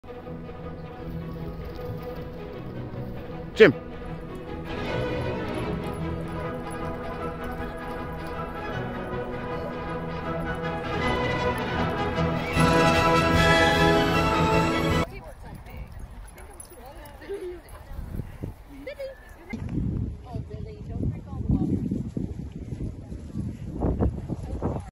Rescue greyhound’s first whirl. Running with whippets
Jim, our retired rescue greyhound likes to be active.

So we signed him up for a session with local whippets.

Although he’s taller than those beautiful sight hounds, he loves running with them.

 I hope you enjoy the video as much as I do